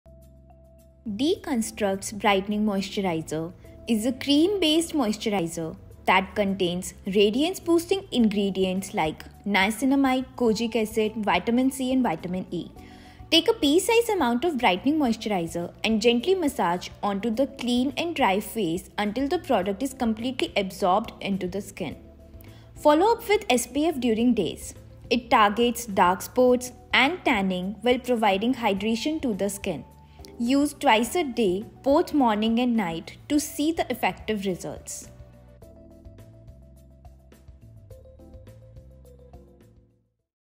How to use Deconstruct's Brightening Moisturizer?
If hyperpigmentation, dark spots, dull skin or acne marks are your skin concerns - then I have the perfect solution for you.
Deconstruct’s Brightening Moisturizer is a cream-based moisturizer that contains radiance-boosting ingredients like Niacinamide, Kojic Acid, Vitamin C & Vitamin E.
 It targets dark spots and tanning while providing hydration to the skin.
Watch this video to know how you can implement it into your skincare routine!